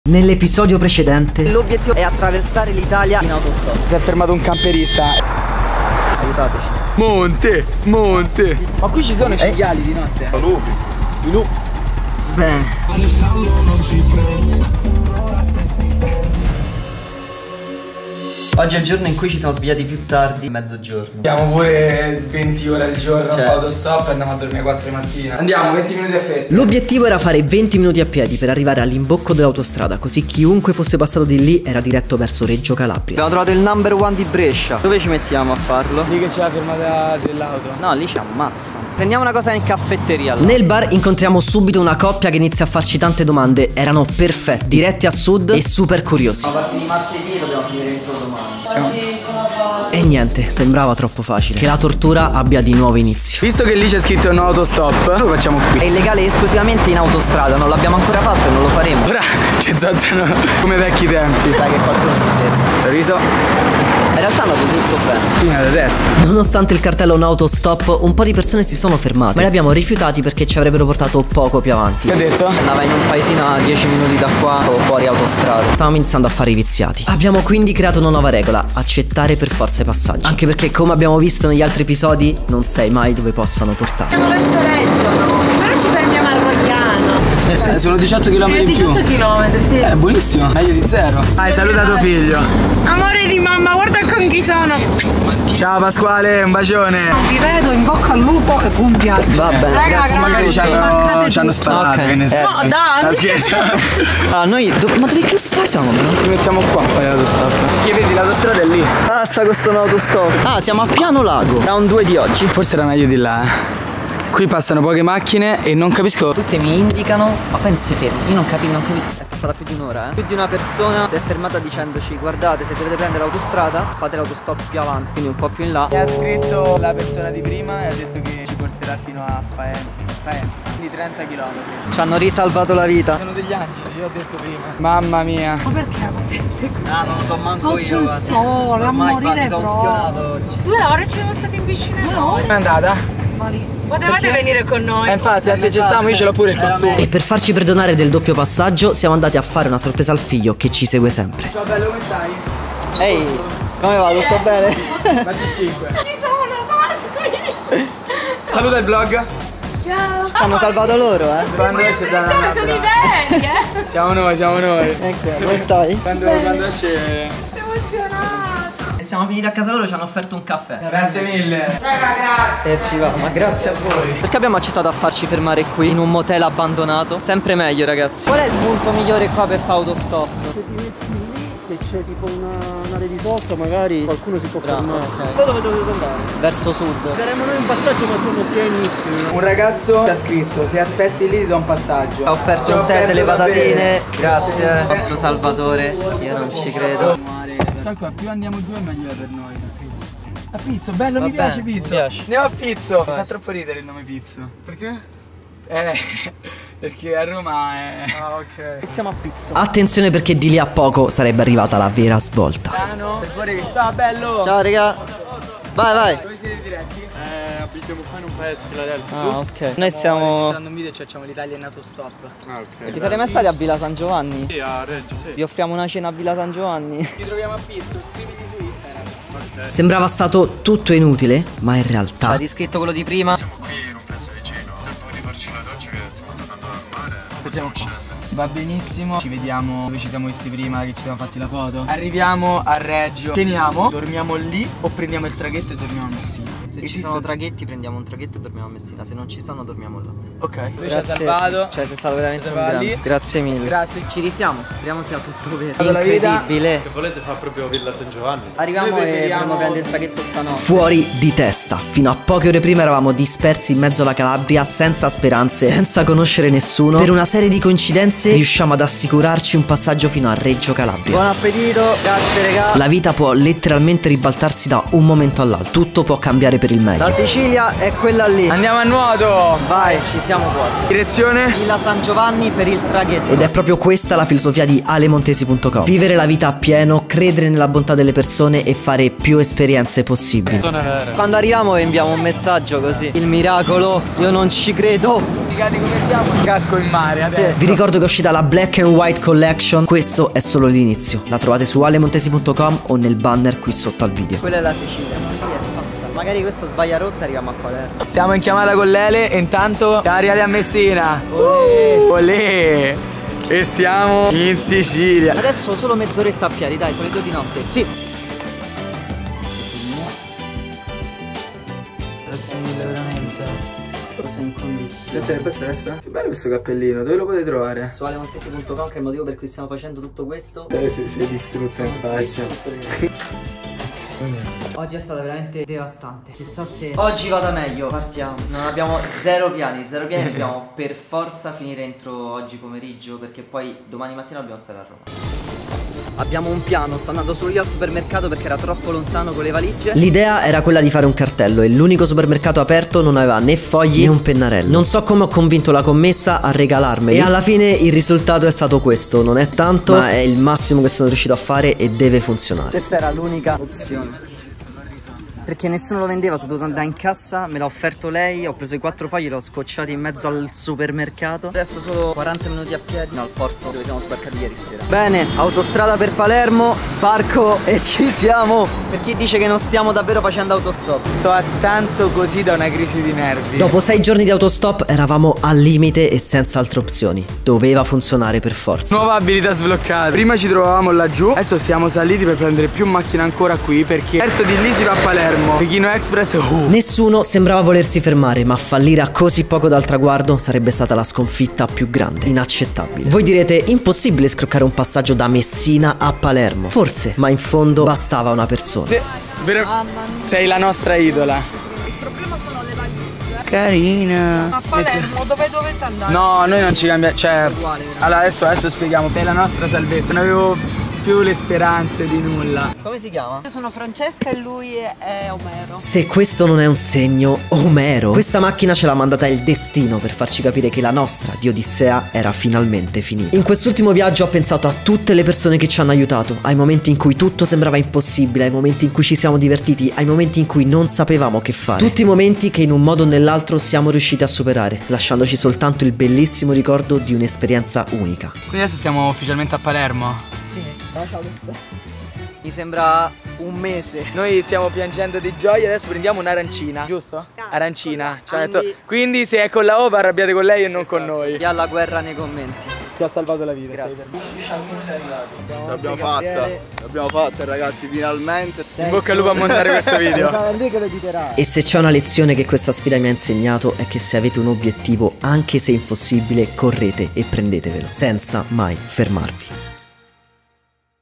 ATTRAVERSO L'ITALIA in AUTOSTOP - Episodio 3
Io e Sespo abbiamo deciso di attraversare l'Italia: da Milano a Palermo in Autostop !Possiamo solo chiedere passaggi e non ci è concesso utilizzare mezzi pubblici, o guidare!

Grazie a SONY per l'attrezzatura!